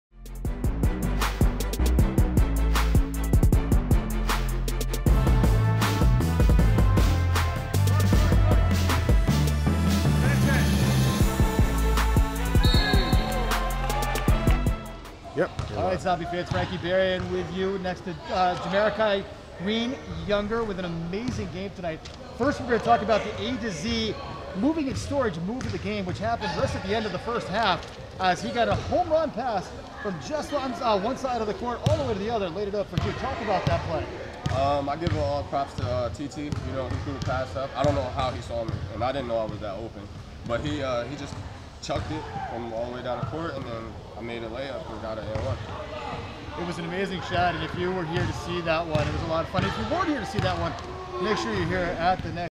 A to Z Move of the Game (5/8/21) - Jahmerikah Green-Younger
In the Zombies second home game against the Plaistow Shockers another new member of the Zombies Jahmerikah Green-Younger had this awesome play near the end of the first half.